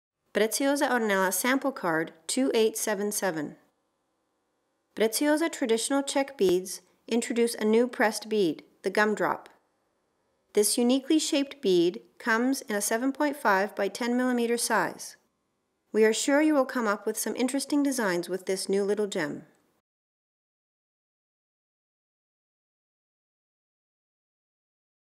PRECIOSA ORNELA SAMPLE CARD 2877
Preciosa Traditional Czech Beads introduces a new pressed bead: the Gum Drop. This uniquely shaped bead comes in a 7.5/10mm size.  We are sure you will be able to come up with some interesting designs with this new little gem!